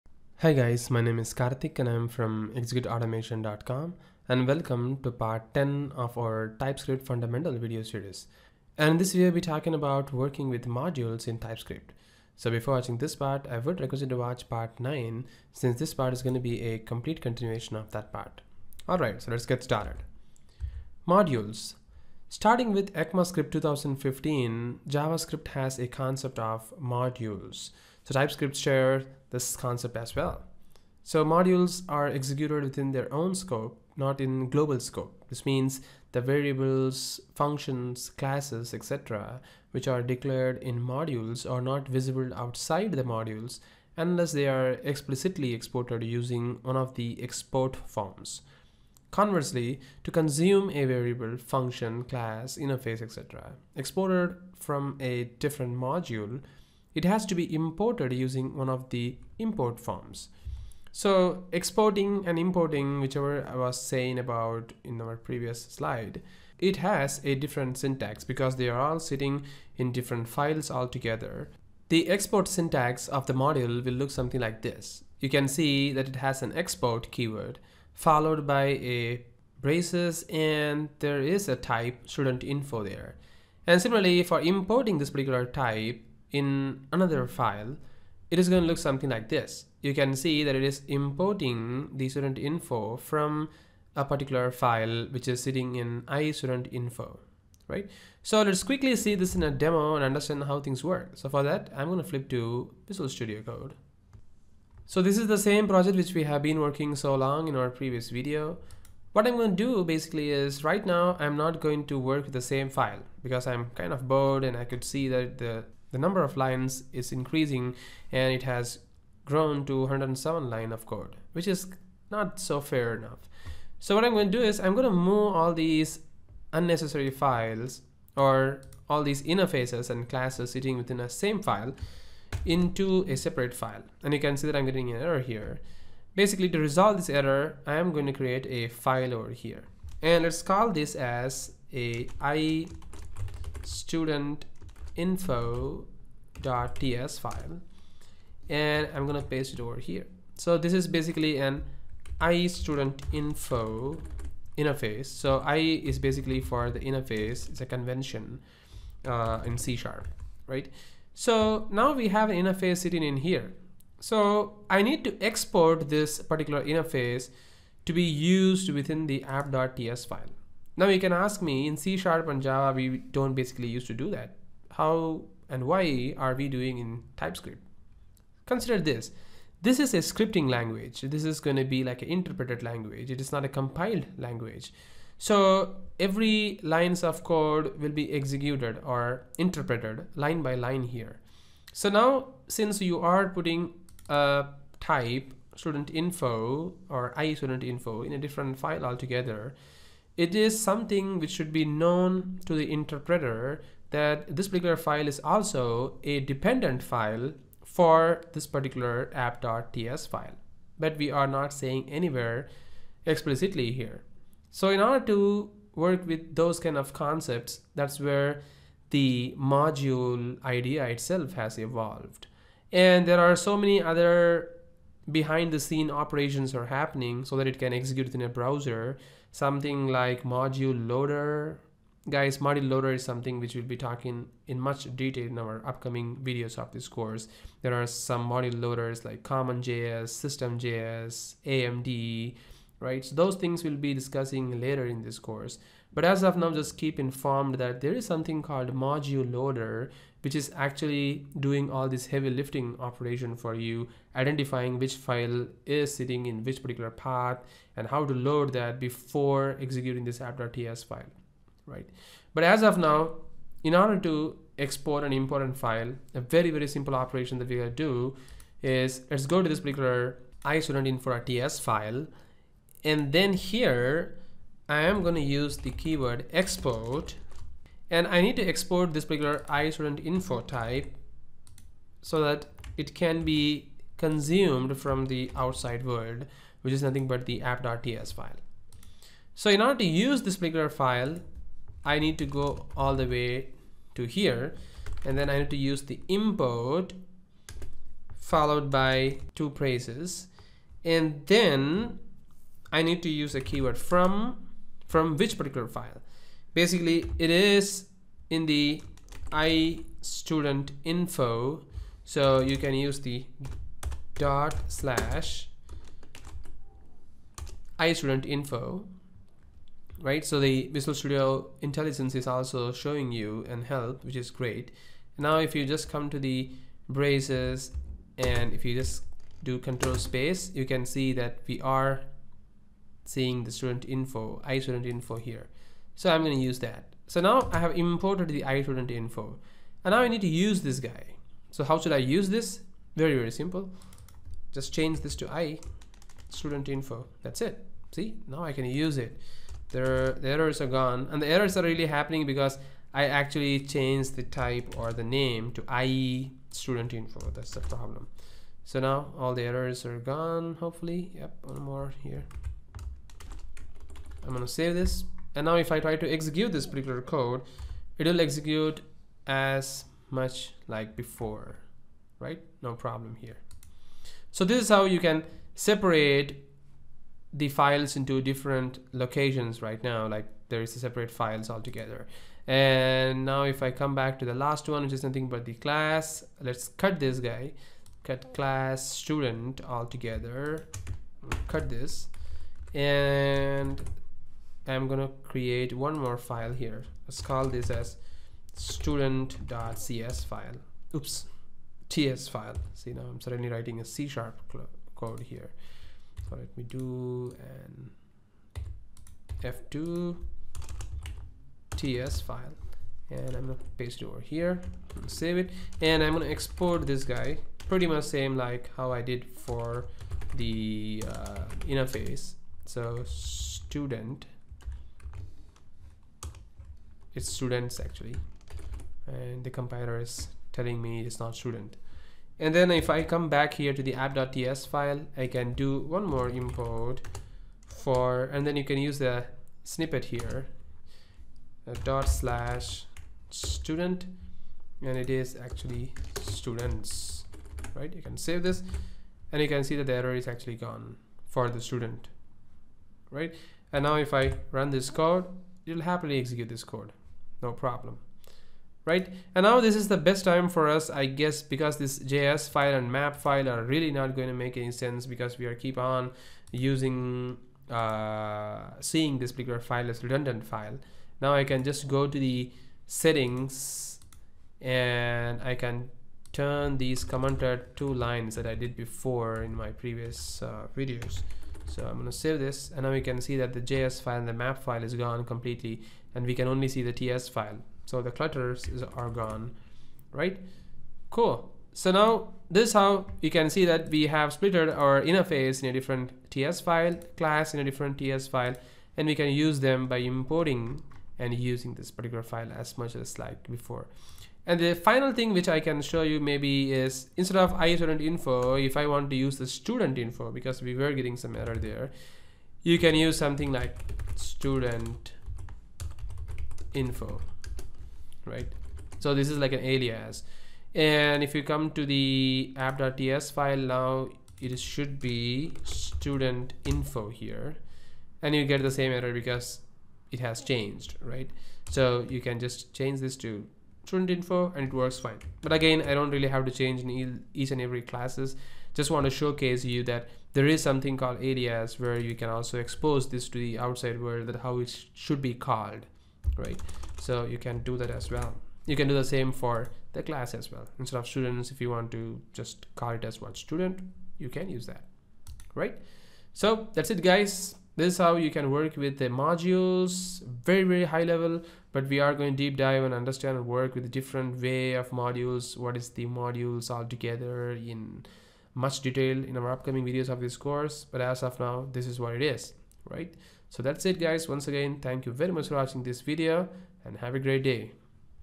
Part 10 - Understanding and working with Typescript Modules
In this video we will Understanding and working with Typescript Modules